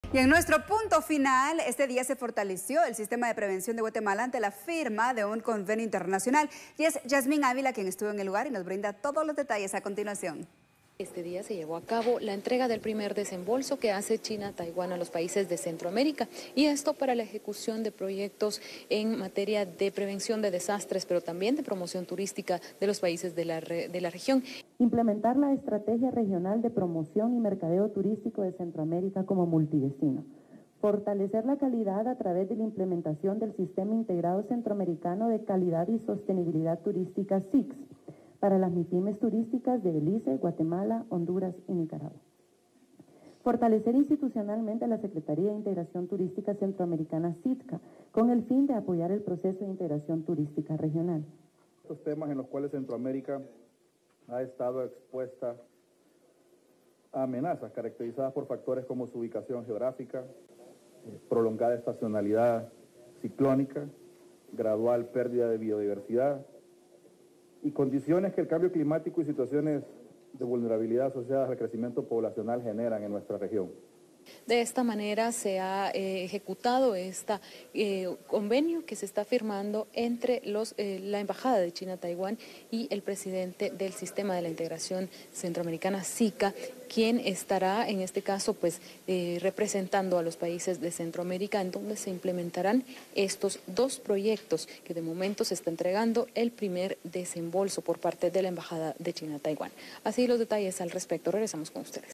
SITCA firma convenio internacional
La jornada de este jueves, se anunció la firma de un convenio internacional entre China y países centroamericanos que apoya el fortalecimiento del Sistema de Prevención de Guatemala. La Secretaría de Integración Turística Centroamericana recibió el primer desembolso por parte de China